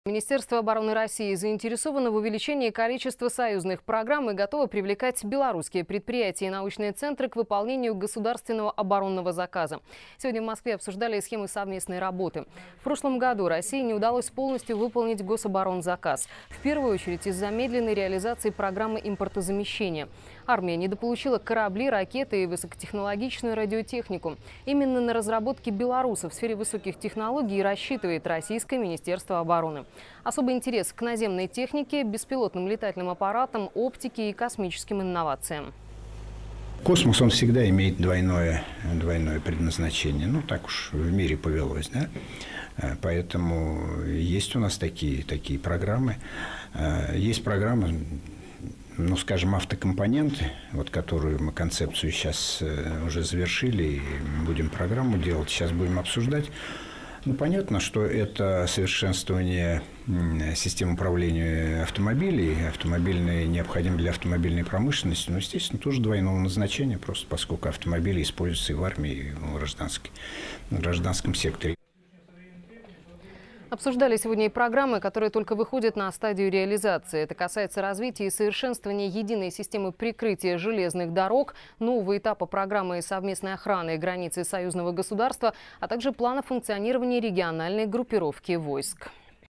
Россия готова привлекать белорусские предприятия к выполнению государственного оборонного заказа
В Москве обсуждали схемы совместной работы. В прошлом году России не удалось полностью выполнить гособоронзаказ. В первую очередь из-за медленной реализации программы импортозамещения. Армия недополучила корабли, ракеты и высокотехнологичную радиотехнику. Именно на разработки белорусов в сфере высоких технологий и рассчитывает российское министерство обороны. Особый интерес к наземной технике, беспилотным летательным аппаратам, оптике и космическим инновациям.